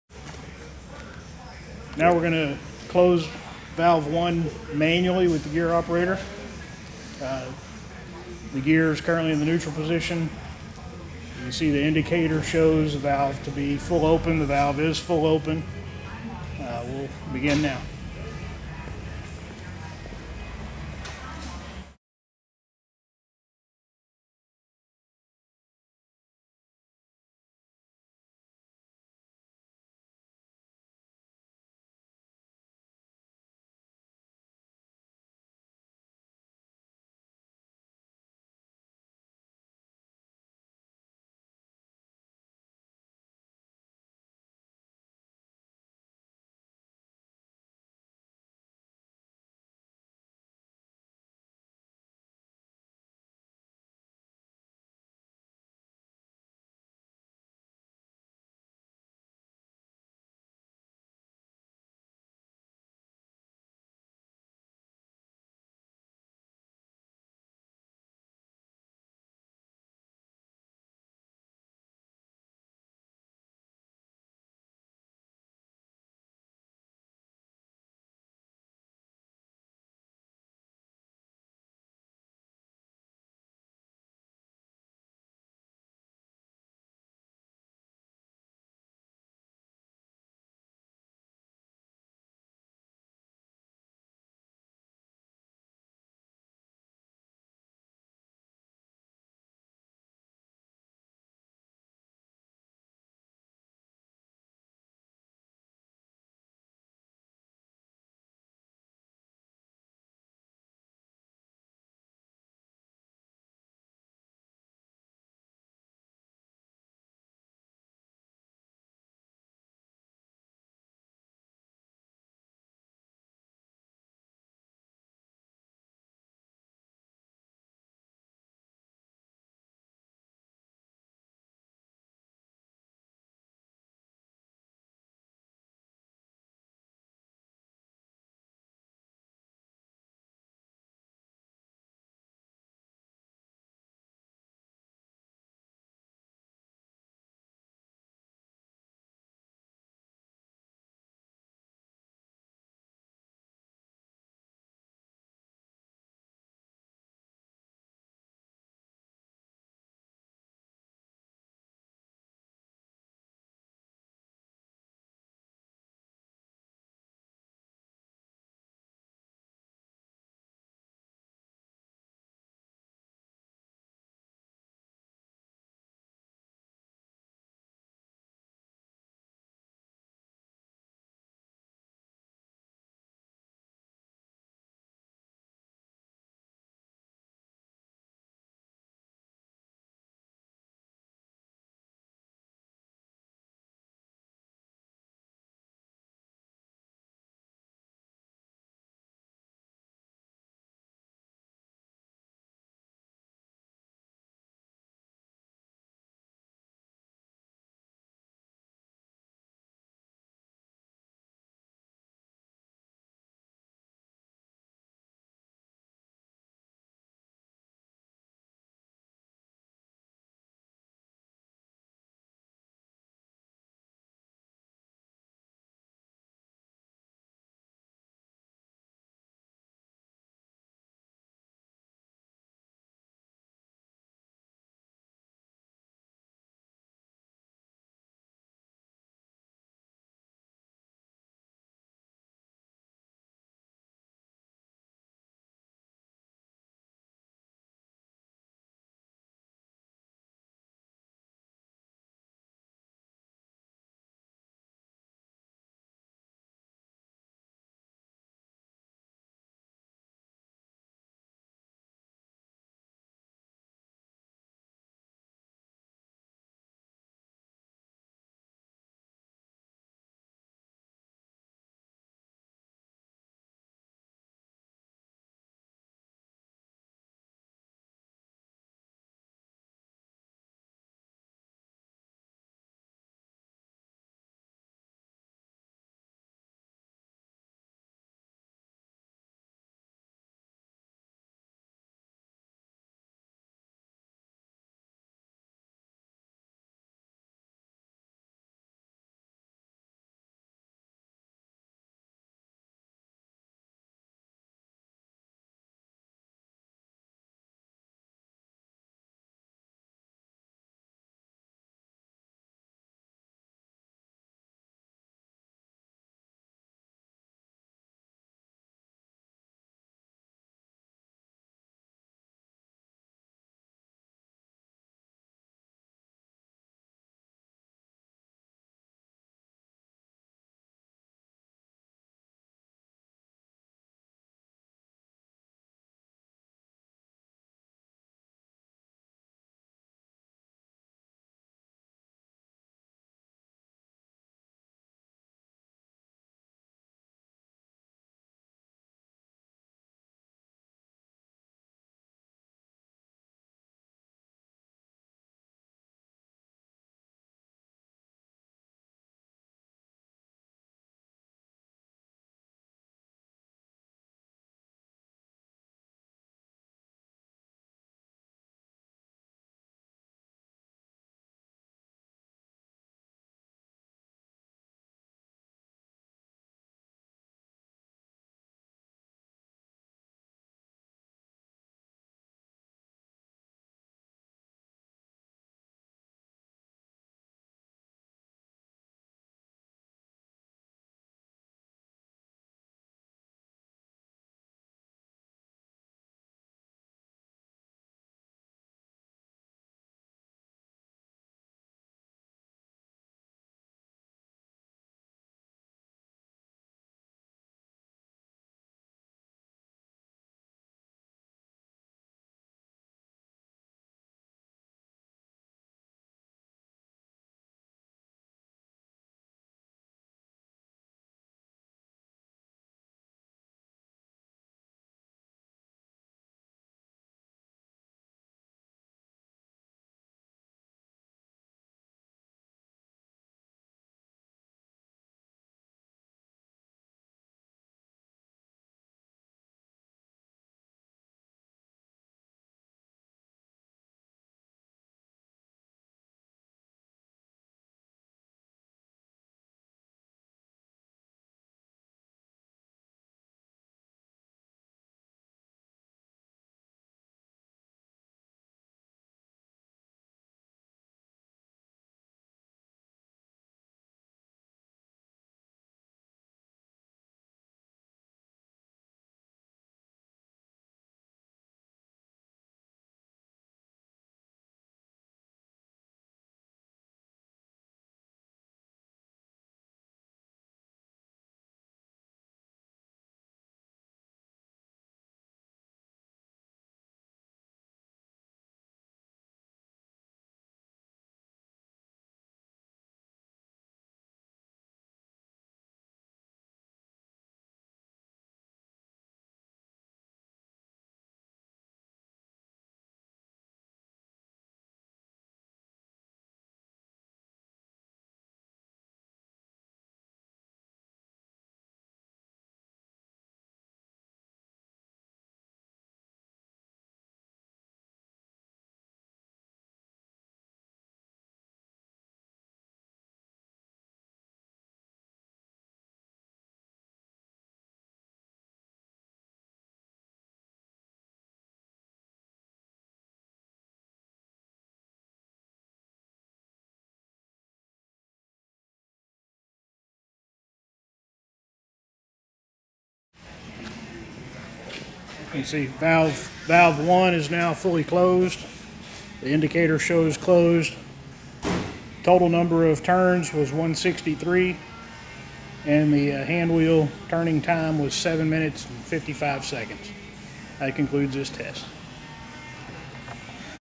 Eii 7 - Full - with ABS Inspector
24" 300 ST3 Ball Valve FAT test performed at Eii Valve Company at their Houston facility.

Eii Valve Company
2115 Pearl Street Houston, TX USA 77029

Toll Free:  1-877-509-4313